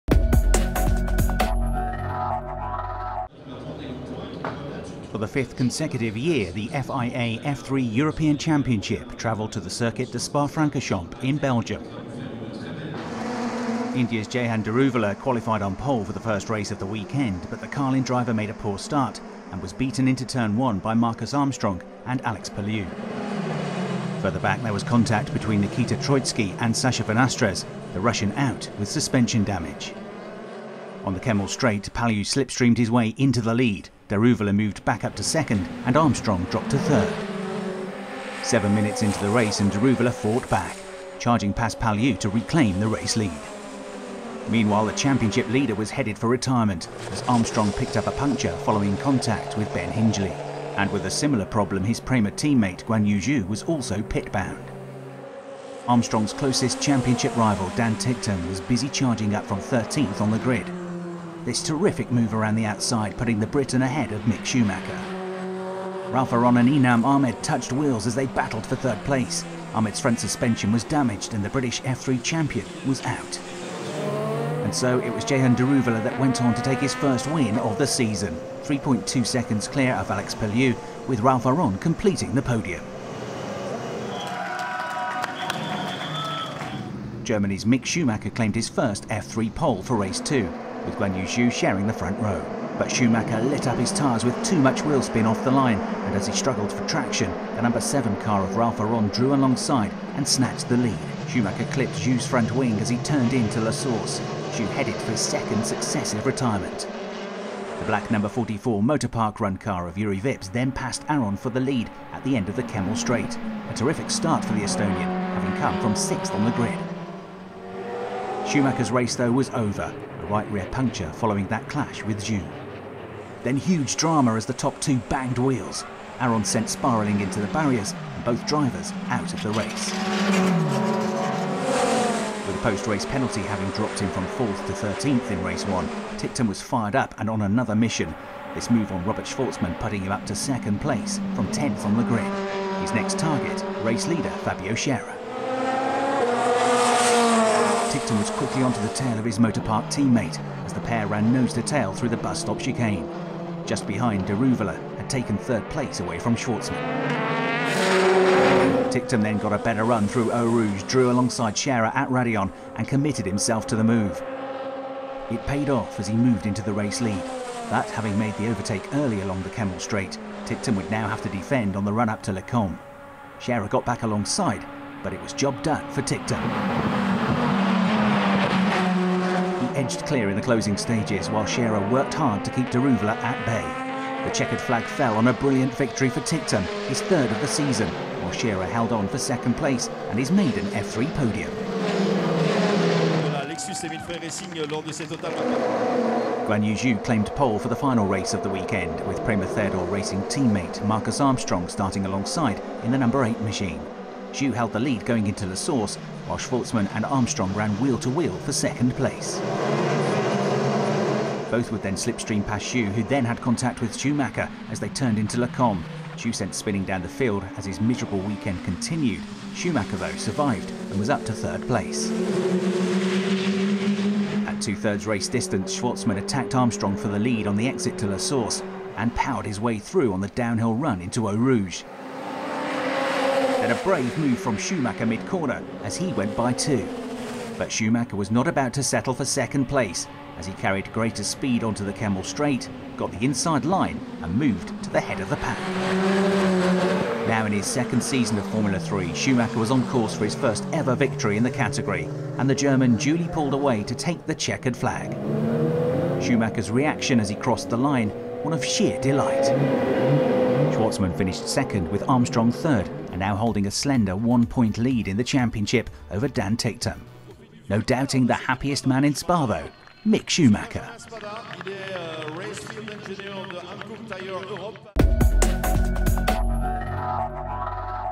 Highlights round 5 at Spa-Francorchamps.
Check out the video summary of all three races at Spa-Francorchamps.